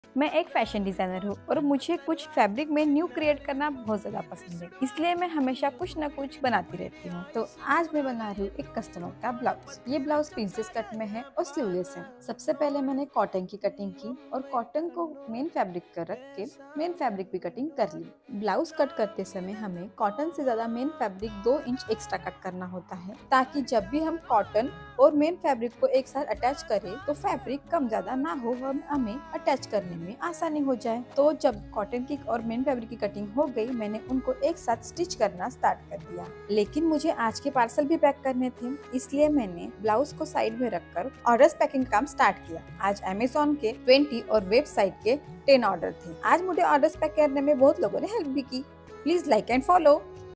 Mini vlog Me and My Small Business 😍🧵👗business
Mini vlog Me and My Small Business 😍🧵👗business fashiondesigner amazonseller diy 👗✂️ Today in the life of our small business: cutting, stitching, and packing! Join us behind the scenes as we transform fabric into beautiful blouses. From precise cuts to expert stitching, every piece is crafted with care and attention to detail. And don't forget about the order packing process – ensuring each package is perfectly assembled before it reaches your doorstep. SmallBusinessLife HandcraftedWithLove"